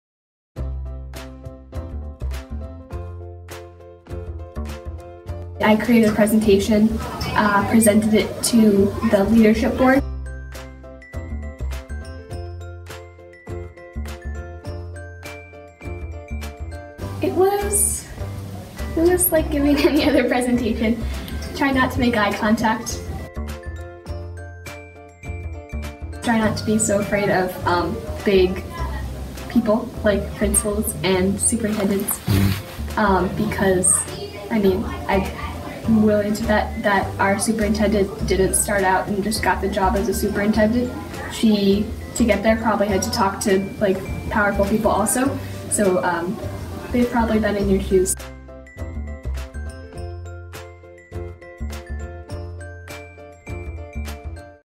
Starting faculty meetings with a student presentation
What does your faculty meeting start with?

Colchester Middle School, in Colchester VT, is trying something new with their faculty meetings. They've started to begin their faculty meetings with student presentations. Let's meet one of the first presenters, 6th grader Moorea, who talks about what it was like to present her research at a faculty meeting, and offers advice to other student presenters.